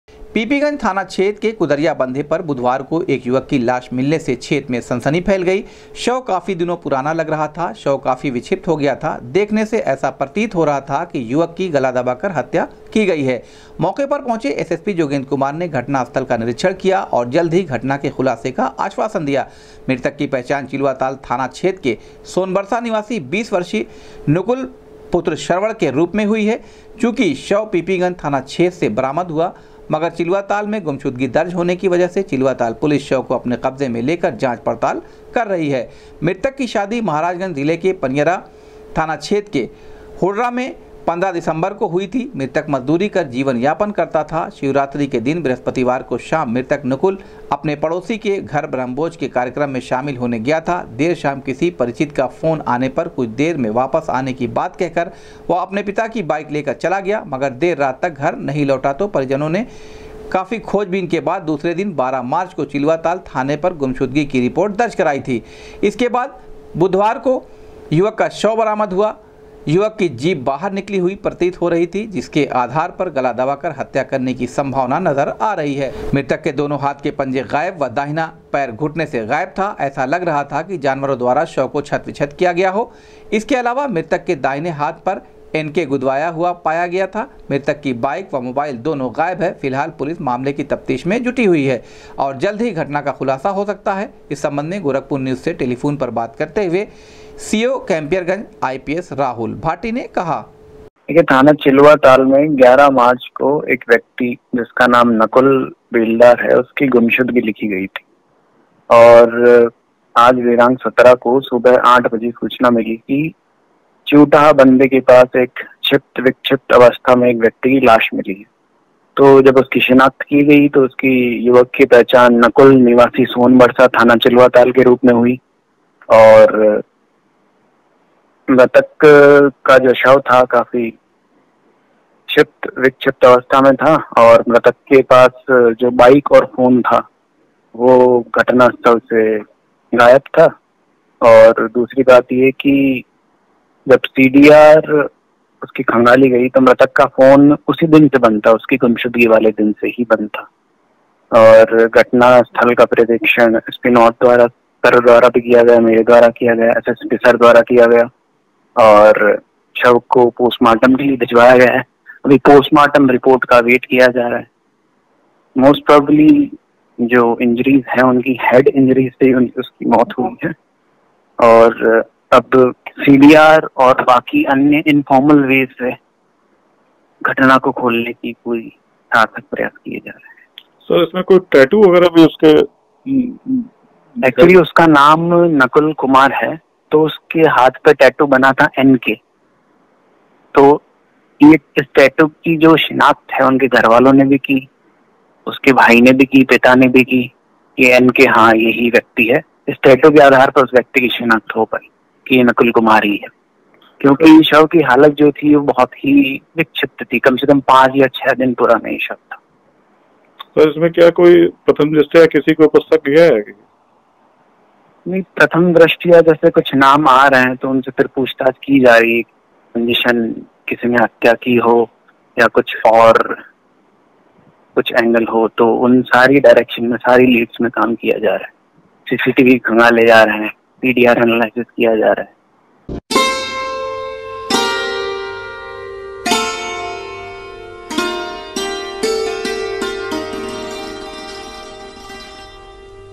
पीपीगंज थाना क्षेत्र के कुदरिया बंधे पर मिली चिलुआताल के युवक की लाश,हत्या का आशंका,मौके पर पहुॅचे
पीपीगंज थाना क्षेत्र के कुदरिया बंधे पर मिली चिलुआताल के युवक की लाश,हत्या का आशंका,मौके पर पहुॅचे एसएसपी ने कहा,जल्द होगा घटना का खुलासा